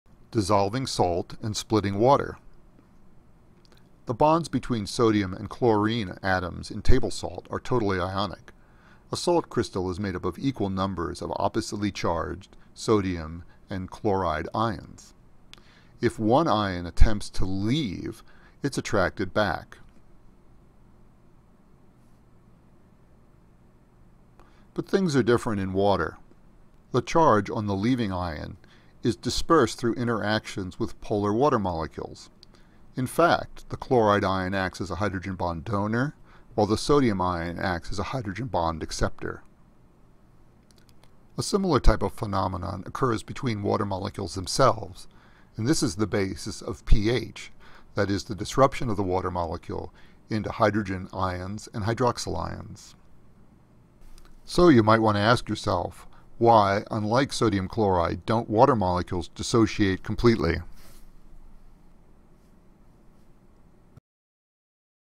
Dissolving salt and splitting water
An short overview of why salt dissolves in water and water comes apart in the hydrogen and hydroxy ions, the basis of pH